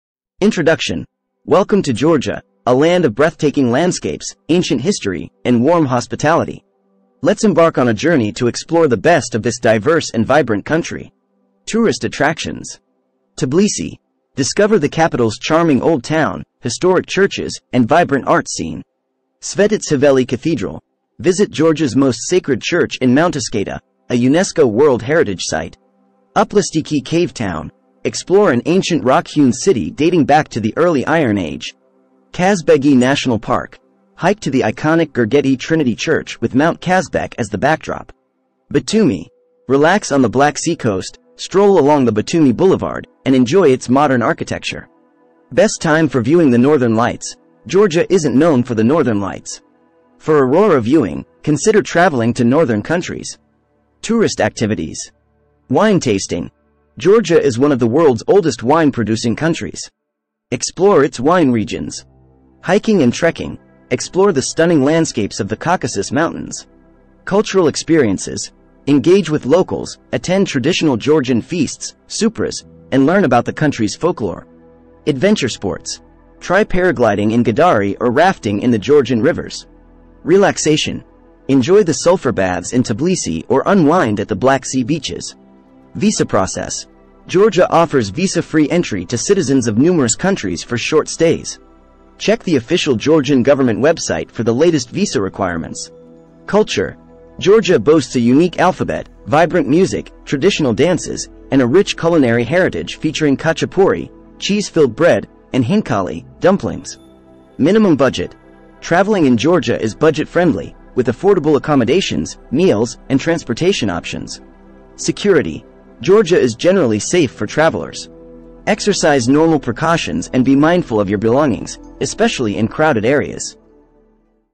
Georgia Where Natural Beauty Meets Rich Culture : Travel Guide
Join us on a mesmerizing journey through Georgia, where natural wonders, ancient traditions, and warm hospitality await. Explore the diverse beauty of this enchanting country with us.